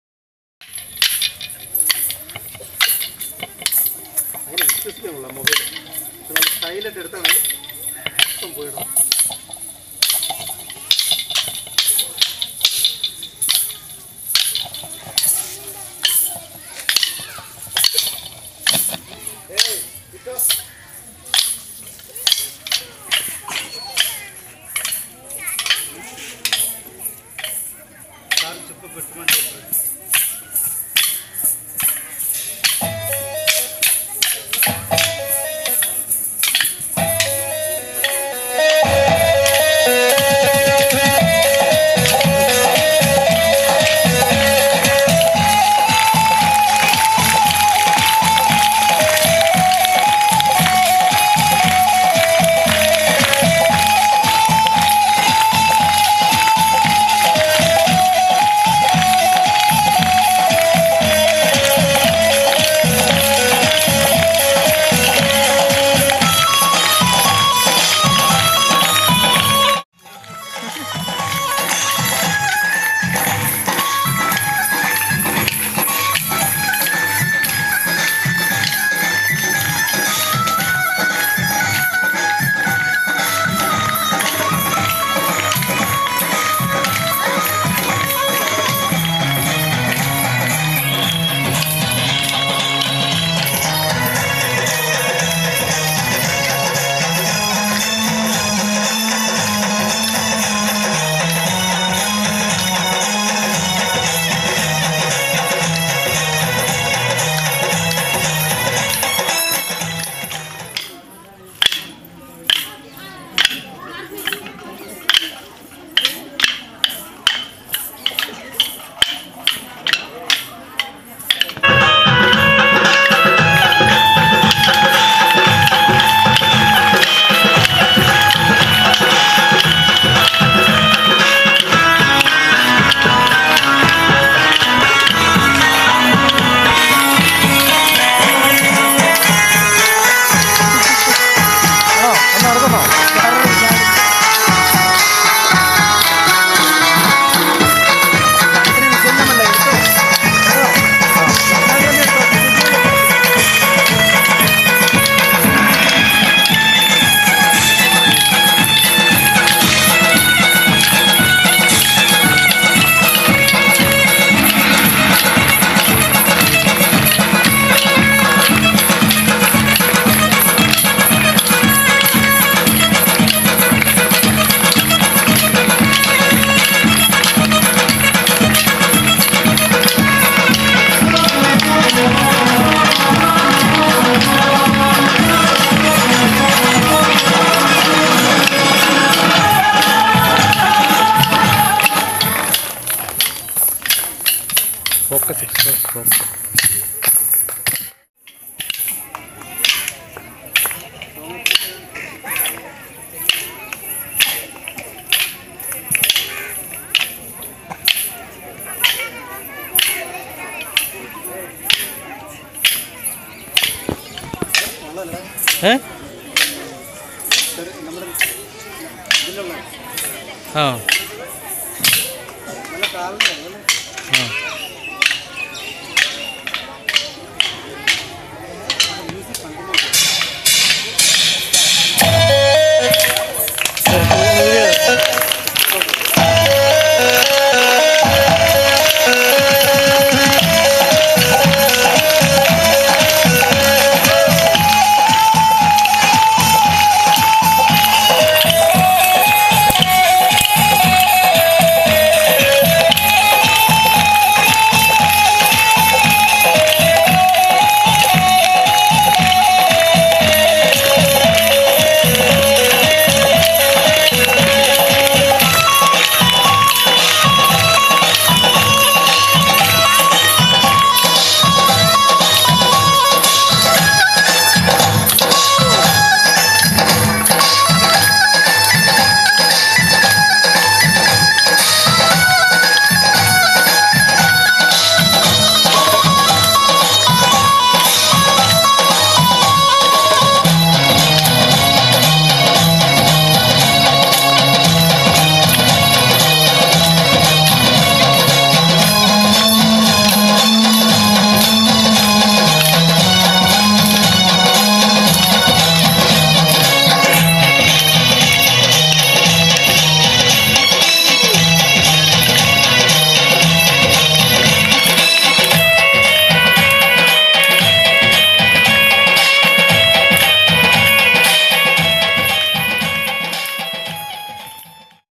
Kolatam -Andhra traditional
Kolatam team TedIam village.
( Greeshma, Roopa Sri, Indu Sri & Bhargavi)